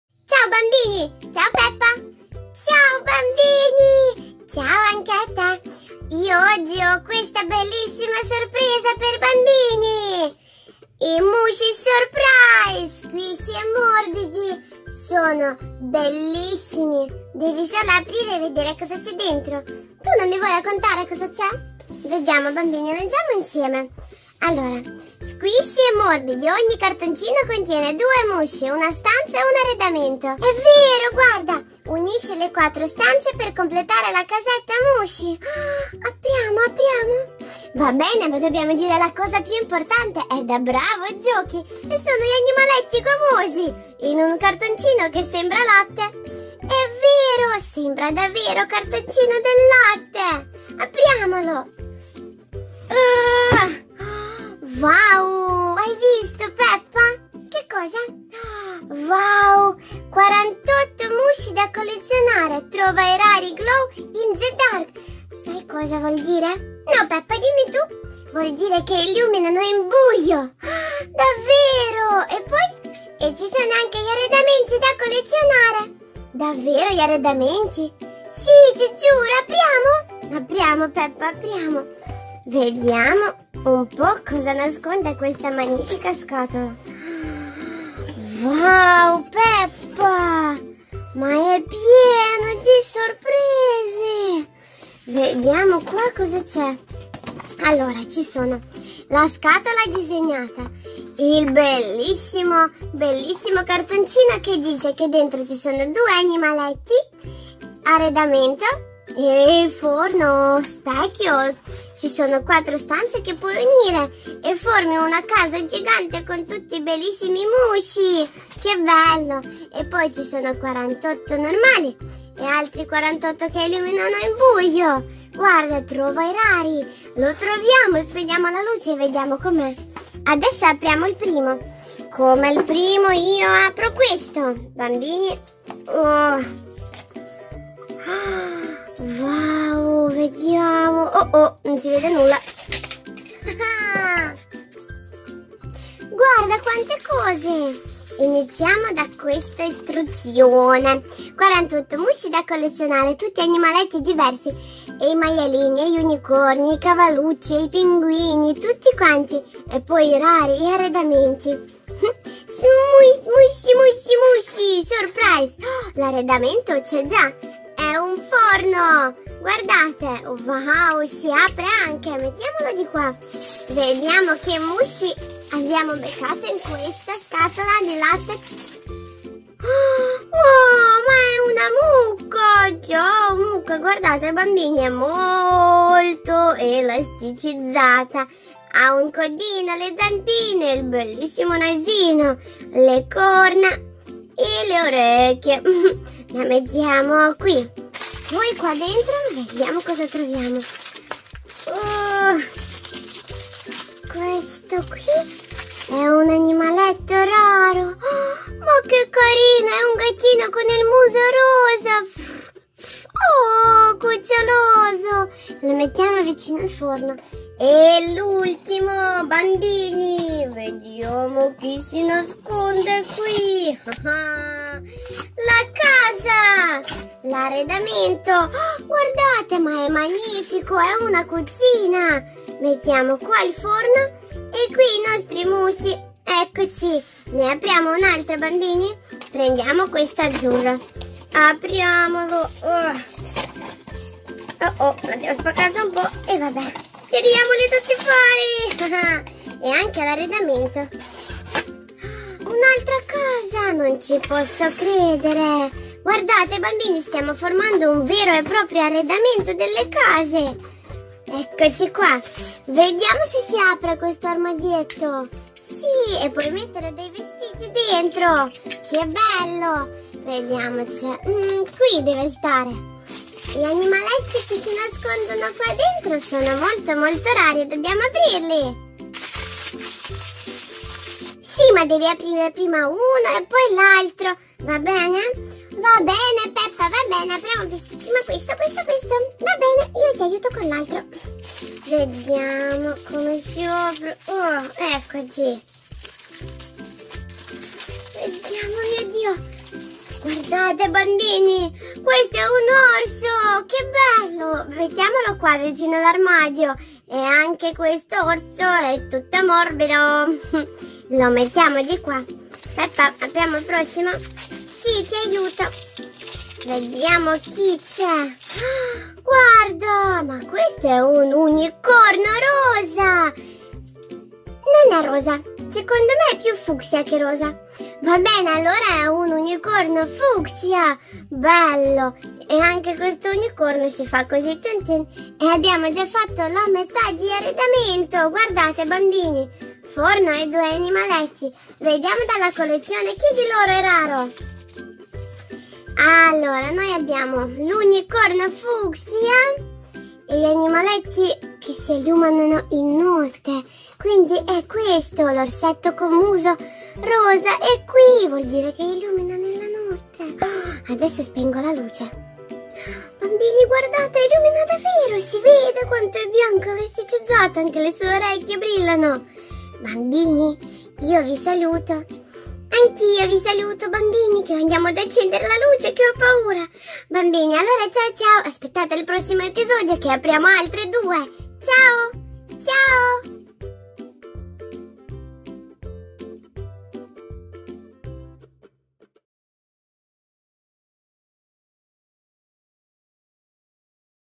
PEPPA PIG gioca con ANIMALETTI MORBIDOSI e cucciolosi Moochies Surprise 😍
ANIMALETTI MORBIDOSI e cucciolosi Moochies Surprise: Completiamo tutte le stanze con tutto arredamento con Peppa Pig!